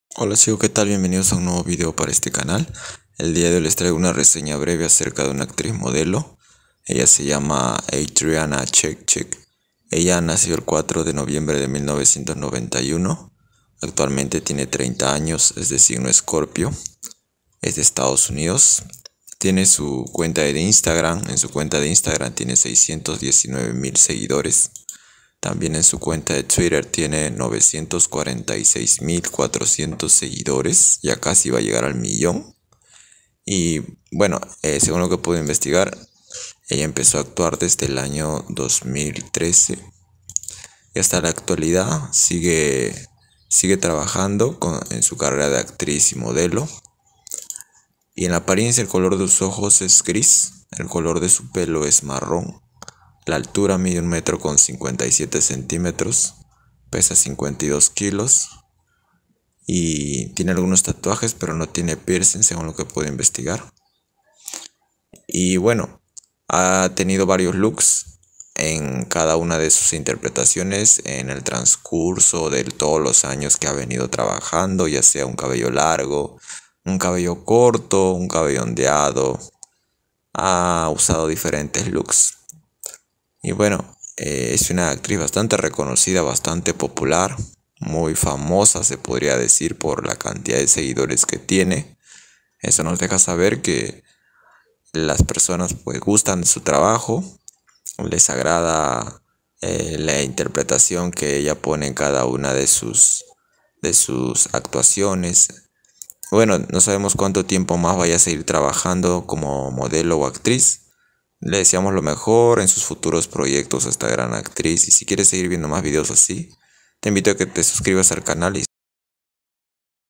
ADRIANA CHECHIK | THE ACTRESS WHO STARTED IN 2013 AND WITH MORE THAN 946 THOUSAND FANS ON TWITTER
In this video I will talk about one of the most famous model actresses in the world, she is well known and has many fans on her social networks.